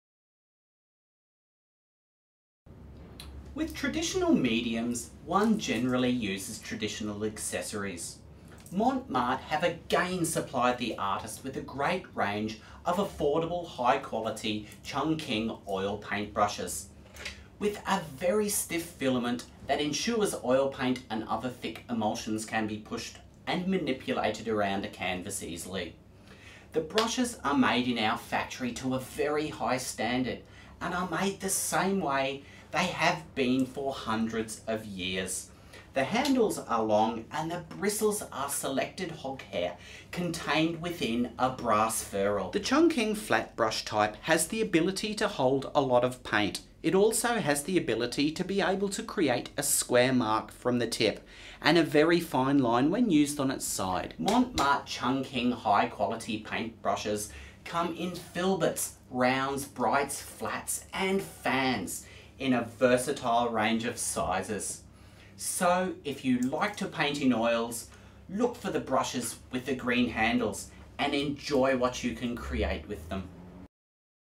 Demo - Artist Brush Chungking Flat - MPB0081, MCG0098-102
Produces long fluid marks.  When used on the point, brush produces fine lines.  Long flexible bristles are good for blending and/or large covering brush strokes.